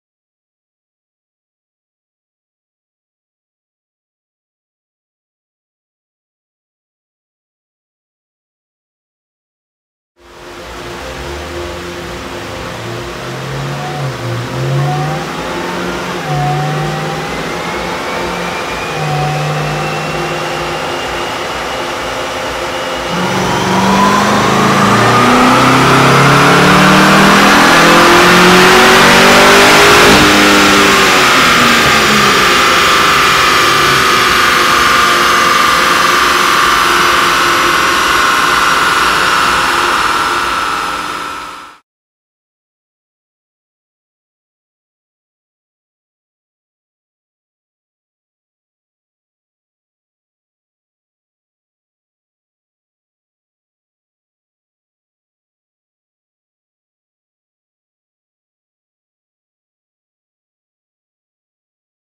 2011 CTS-V Sport Wagon Dyno Test
Hennessey Performance dyno tests a stock 2011 Cadillac CTS-V Sport Wagon equipped with an automatic transmission.